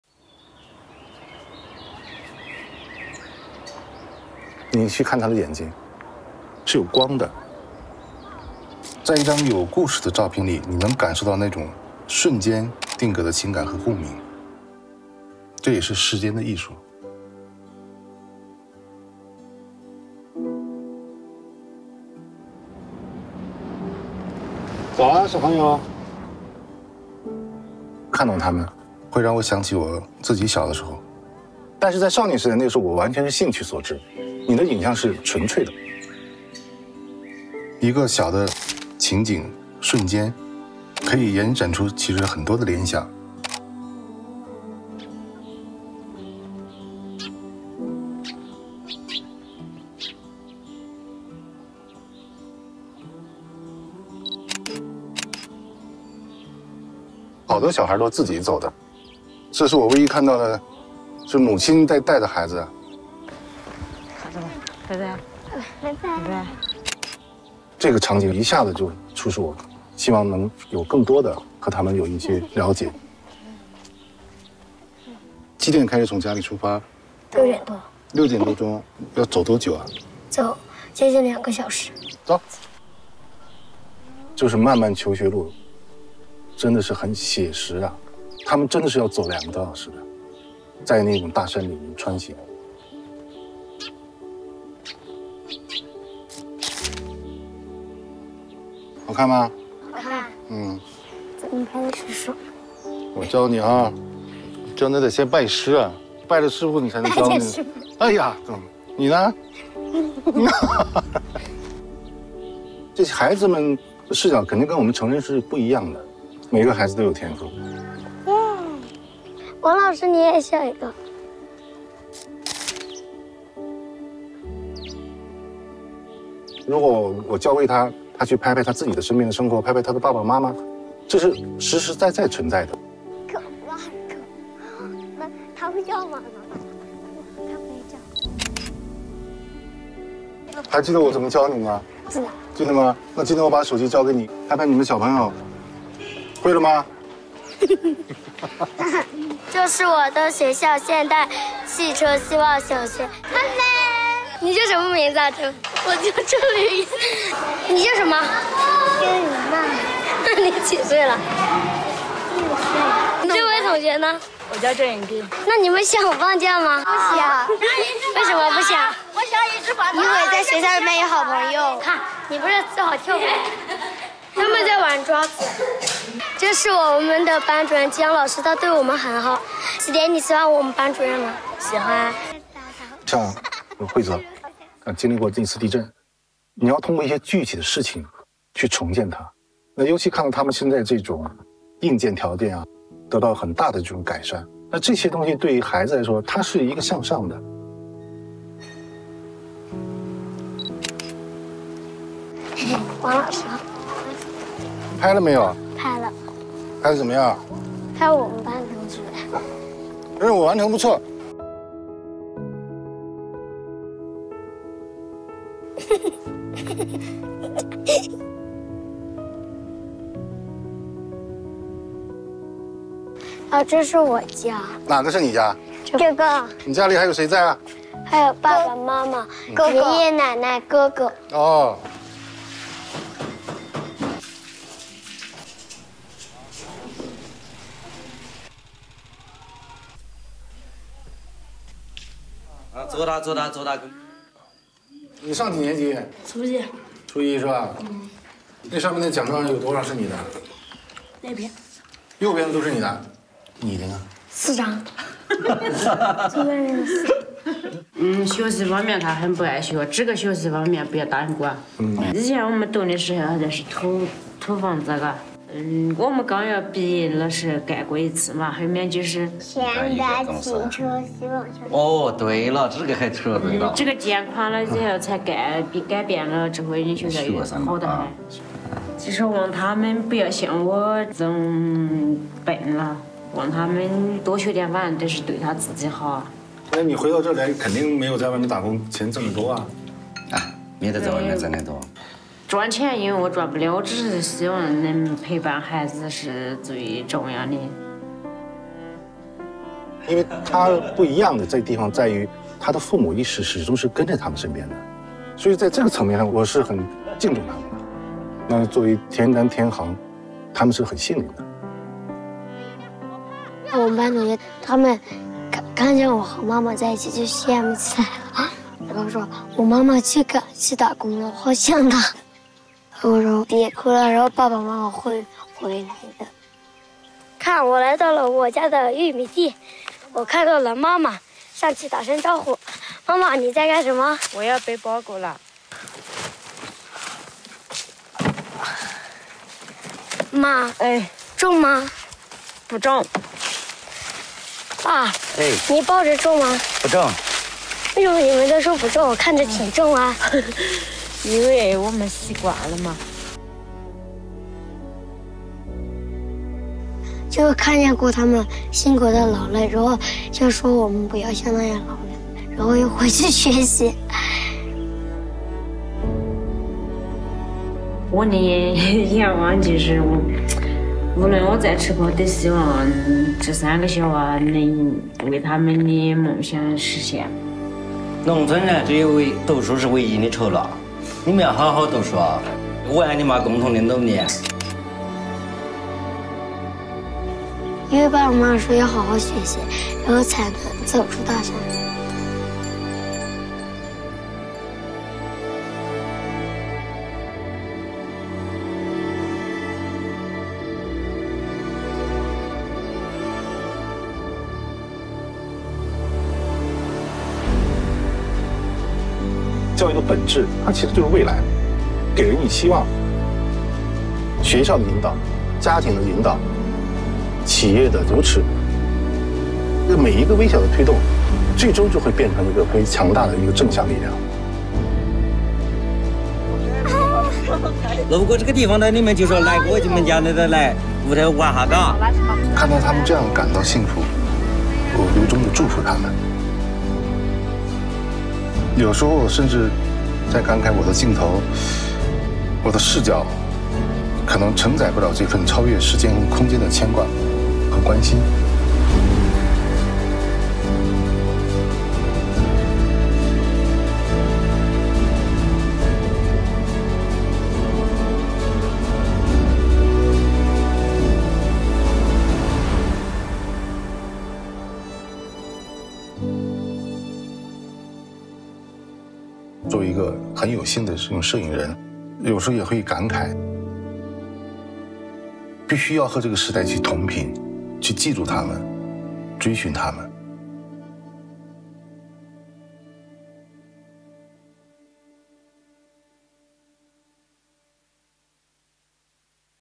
현대자동차그룹 중국 진출 20주년 다큐멘터리 with 디스커버리 – 운남 Dream Move편
2022년은 현대자동차그룹이 중국 진출 20주년을 맞는 해입니다.

중국 환경 보호와 사회 공익에 힘써 온 현대자동차그룹은
8년 전 지진 피해를 입은 중국 운남성 훼이저현 내 학교 건물을 재건축했습니다.
‘현대자동차 희망초등학교’로 불리는 이곳은 운남성에 어린이들의 꿈과 희망을 지켜주고 있습니다.

중국 촌락의 변화를 기록하는 사진작가 왕징춘이 바라본
티엔단, 티엔항 자매의 하루와 운남성 친구들의 아름다운 미소를 영상으로 만나보세요.

본 다큐멘터리는 벽지 학교에 교육 시설을 기부하여 더 좋은 교육환경과 아름다운 미래를 만들어 주는
현대자동차그룹 HYUNDAI DREAM MOVE 공익 프로젝트를 배경으로 제작되었습니다.